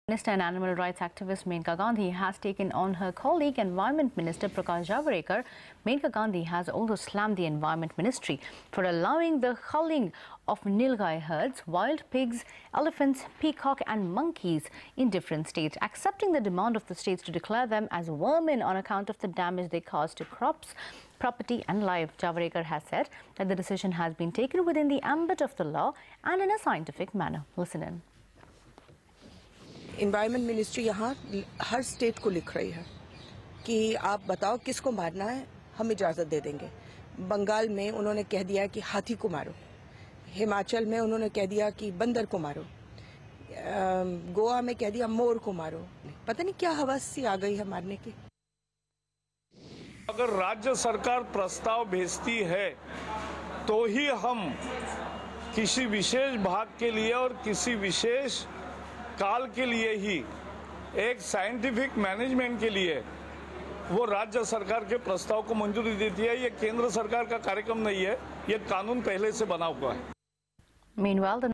Maneka Gandhi Locks Horns With Green Minister
Maneka Gandhi locks horns with Green Minister over culling of animals.